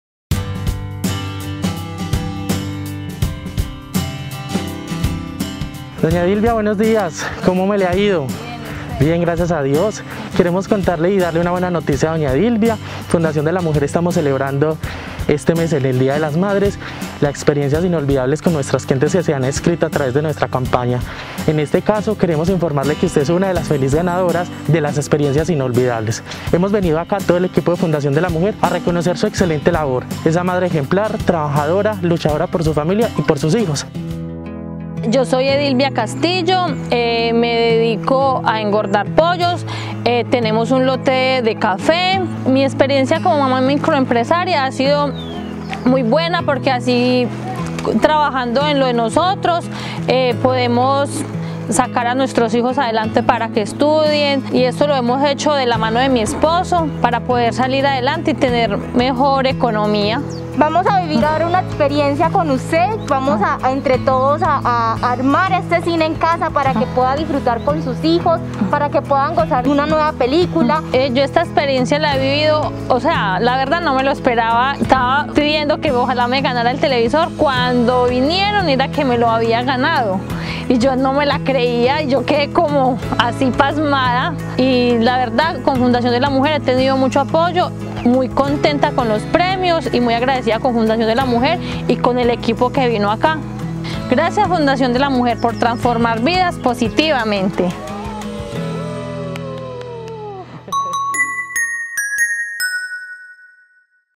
Edilbia, una mamá invencible
¡Estuvimos en Ciudad Bolívar, Antioquia, para cumplir el sueño de nuestra cliente! Conozca más de Edilbia Castillo, una mamá invencible que vivió una experiencia inolvidable.